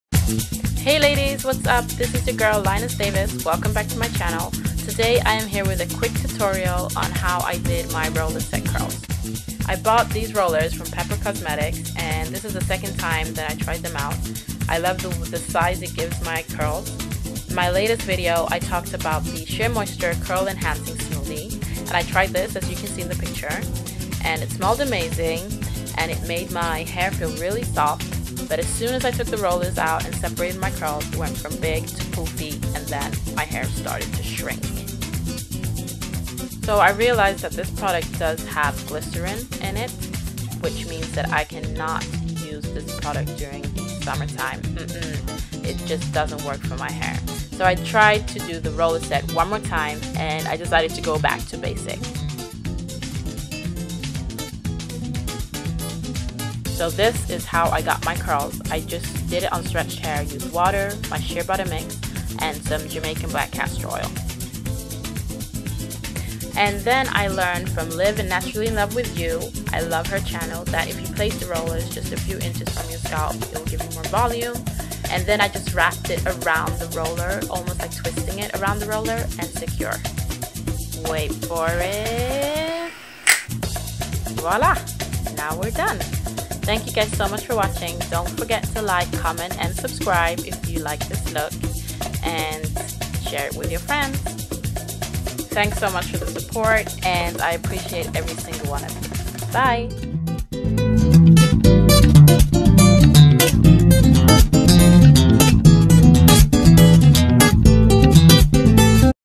Roller setting natural hair | Lioness Davis
Hey, this video is of my first successful roller set that lasted three days and a quick review of the Shea moisture curl enhancing smoothie.
Shea mix: raw shea butter (whipped) Argan oil, olive oil & Jamaican black castor oil.